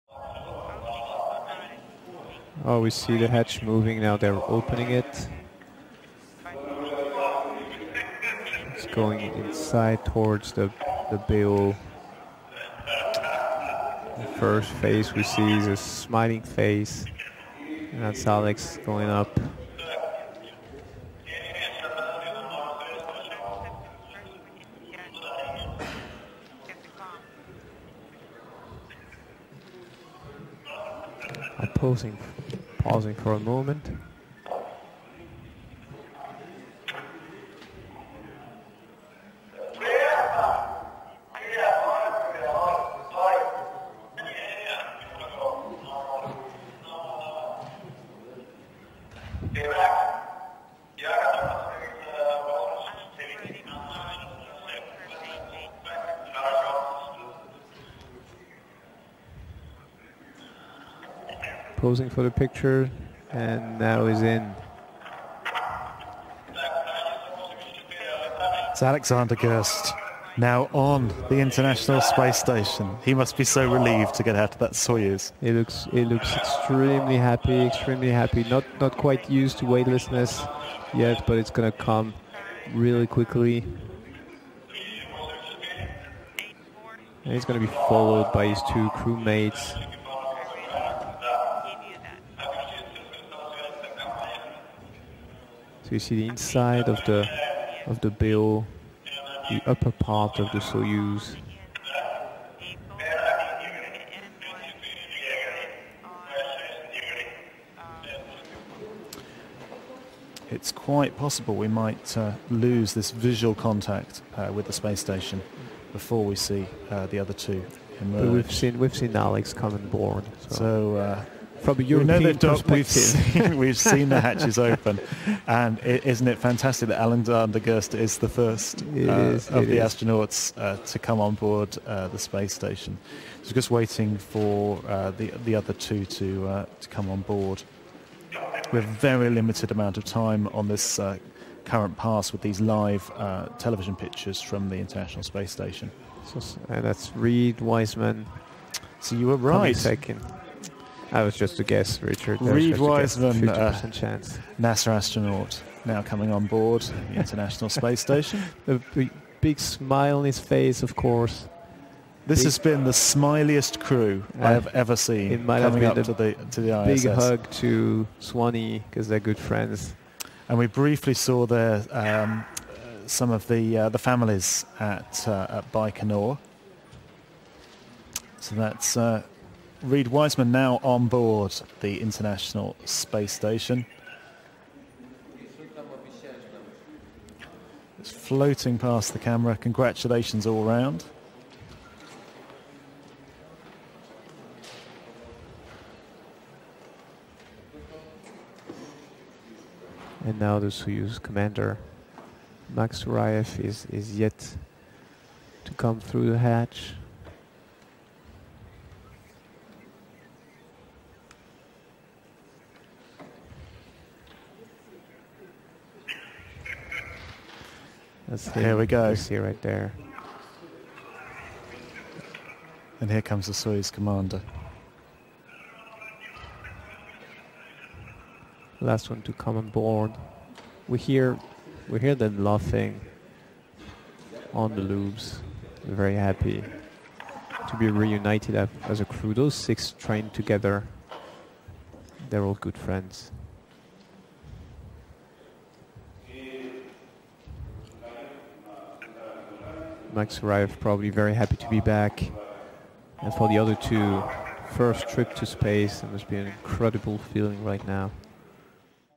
Alexander Gerst enters the Station
Edited highlights of the Expedition 40/41 crew, ESA astronaut Alexander Gerst, NASA astronaut Reid Wiseman and Roscosmos commander Maxim Suraev as they enter the International Space Station, 29 May 2014. For Alexander and Reid it was their first time in space, Maxim had already spend 169 days in space.

The crew will stay on the International Space Station for six months and perform over 100 experiments during Alexander's Blue Dot mission.